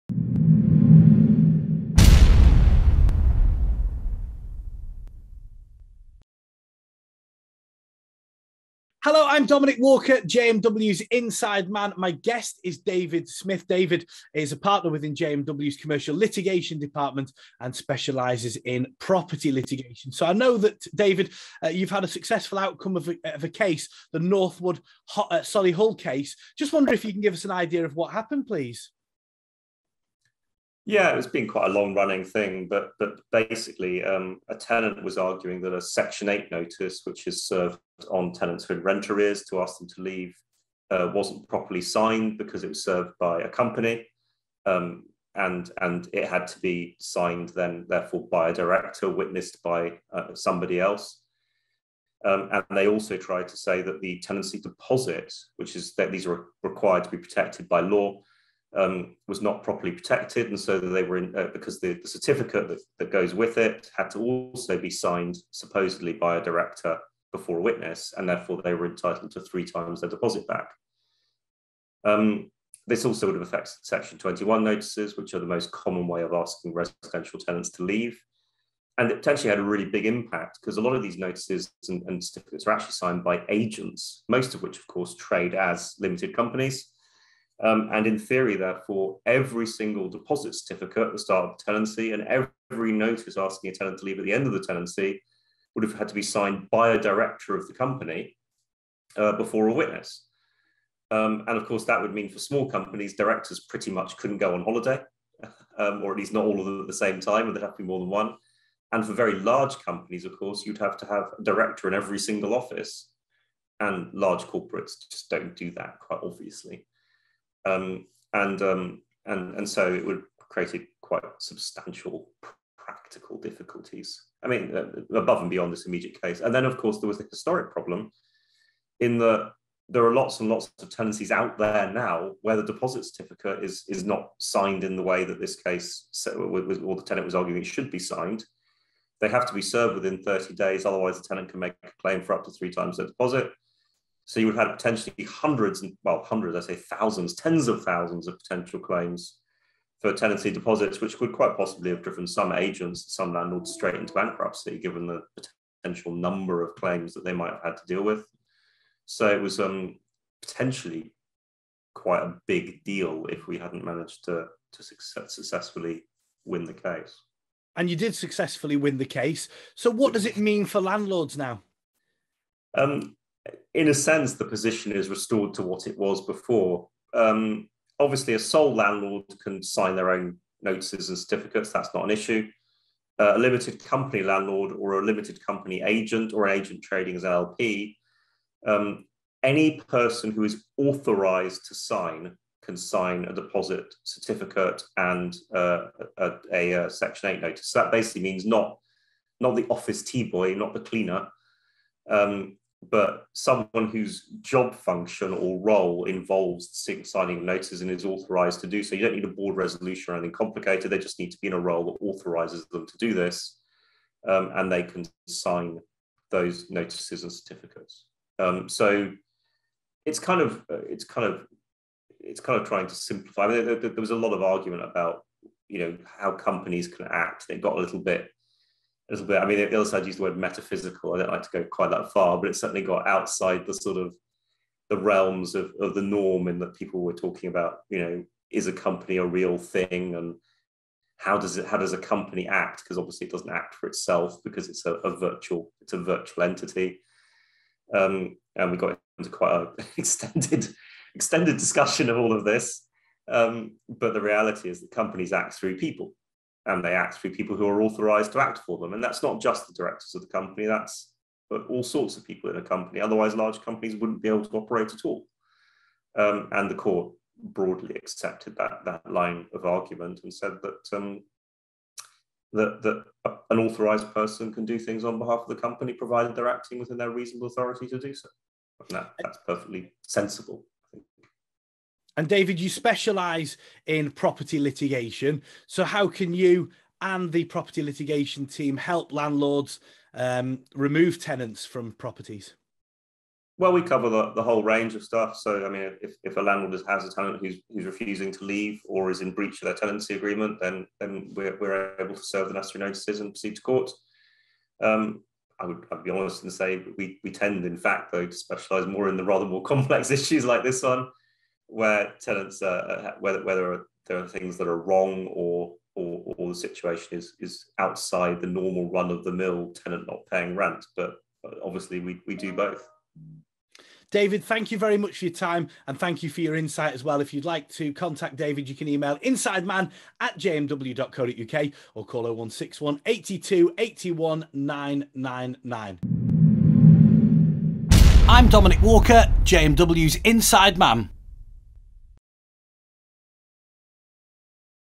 Court of Appeal judgment means that an authorised employee can sign notices or deposit certificate
A recent decision handed down by the Court of Appeal means that an authorised signatory can sign a section 8, section 21 or tenancy deposit certificate.

The Solicitor who acted for the Solihull branch of Northwood, David Smith explains what happened and what it means for agents in the future.